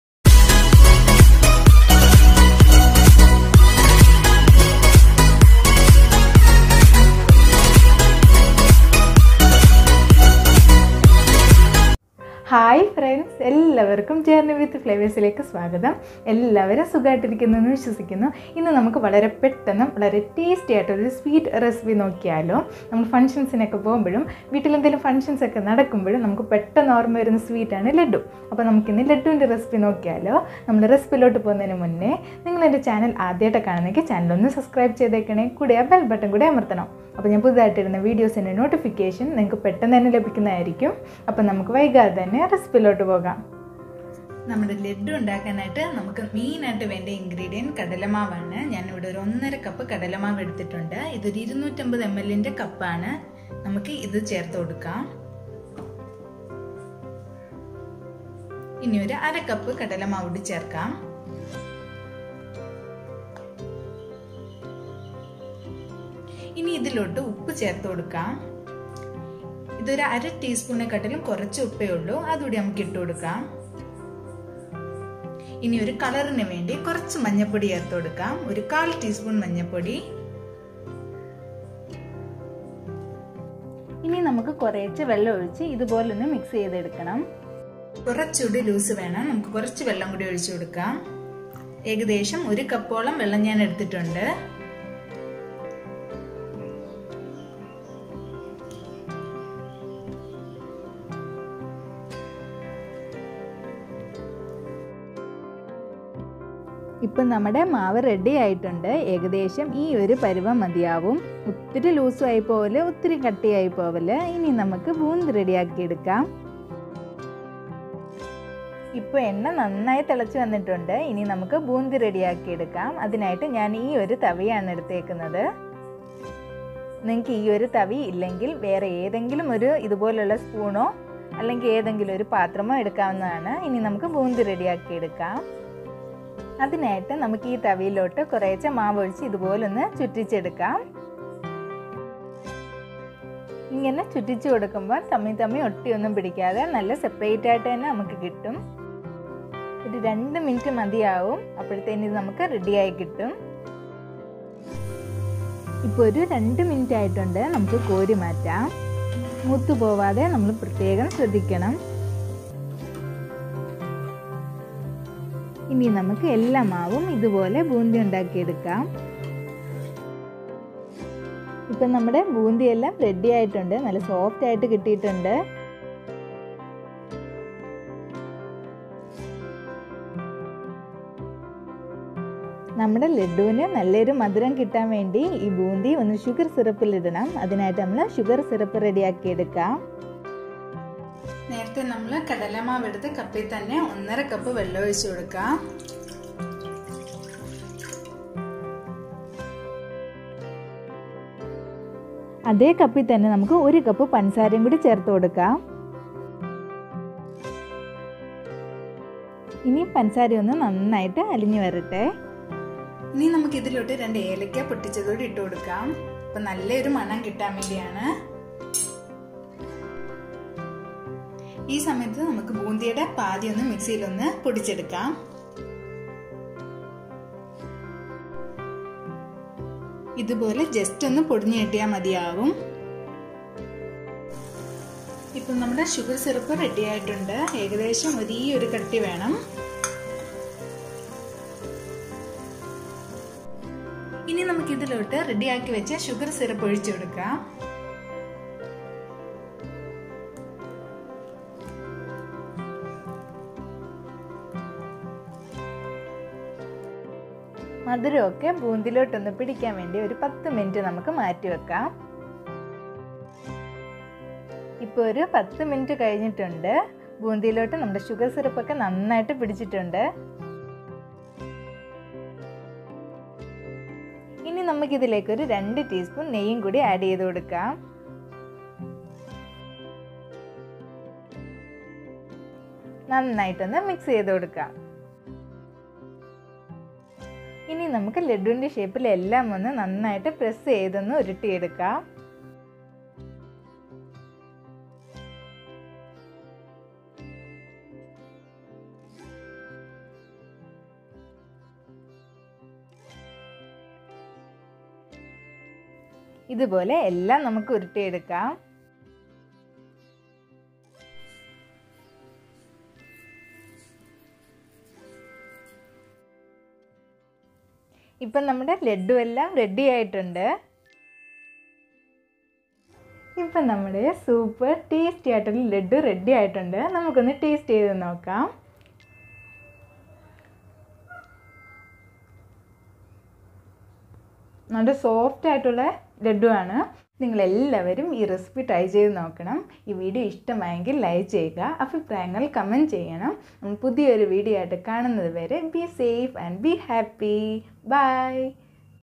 How to make Laddu || ലഡ്ഡു||Laddu Recipe in Malayalam
This video is about the preparation of very tasty and soft laddu in short time. The ingredients are listed below:
Besan floor           -1+1/2cup
Salt                         -1/2tsp
Turmeric Powder-1/4tsp
Water
Oil                           - as required
Cardamom           -2nos
Sugar                     - 1 cup
Please go through the video for preparation
some of our video links are pasted below we hope you will like it. please watch share and support thank you.